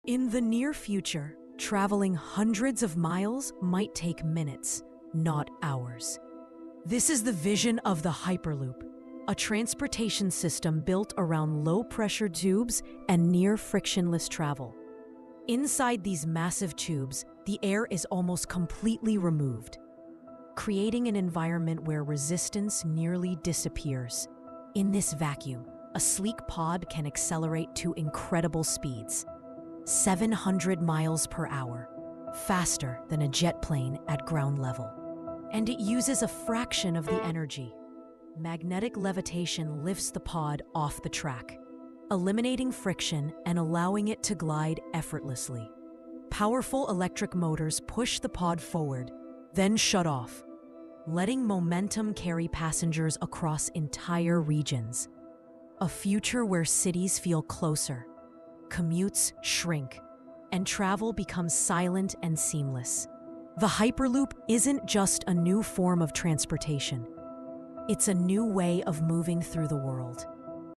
Hyperloop The 700 mph Future of Transportation
The Hyperloop is a proposed ultra-fast transport system designed to move people and cargo at nearly 700 mph using low-pressure tubes and magnetically levitating pods.
With almost zero air resistance and minimal energy consumption, it could shrink commutes, connect cities, and redefine the future of high-speed travel.

This cinematic tech breakdown shows how the Hyperloop works—and why it could revolutionize the way we move.